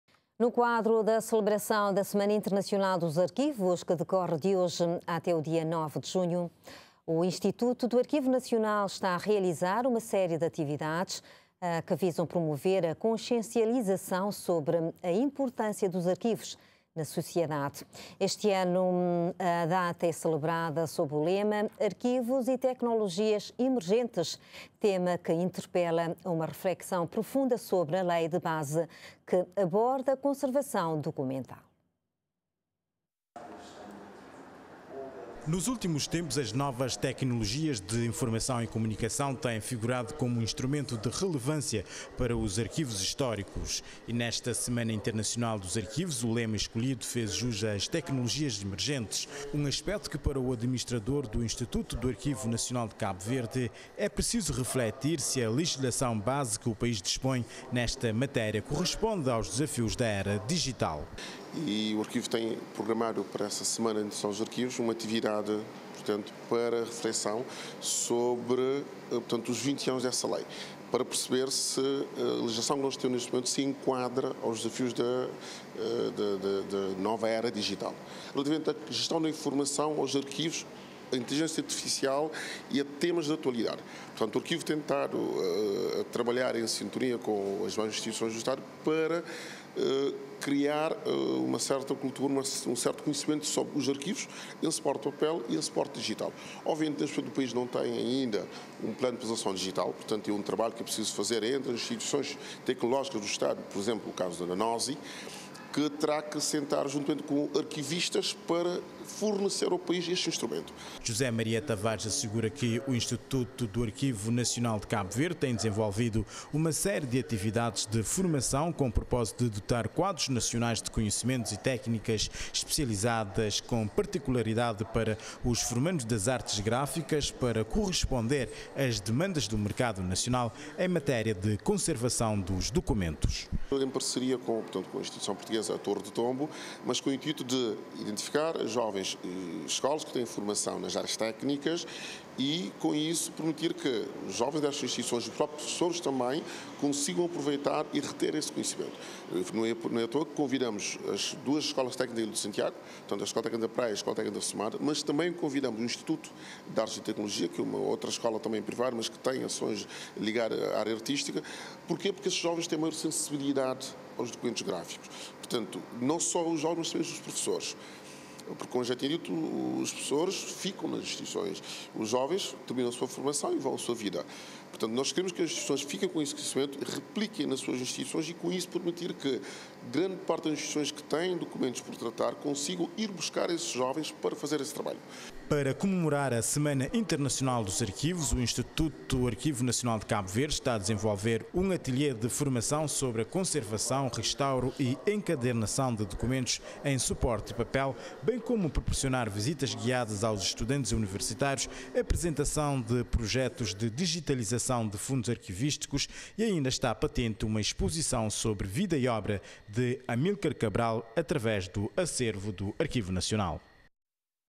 IANCV promove workshop sobre conservação e restauro de documentos para técnicos e estudantes
IANCV promove workshop sobre conservação, restauro e encadernação de documentos dirigido a técnicos e estudantes de artes gráficas
RTC Online. Aqui, você está em Cabo Verde!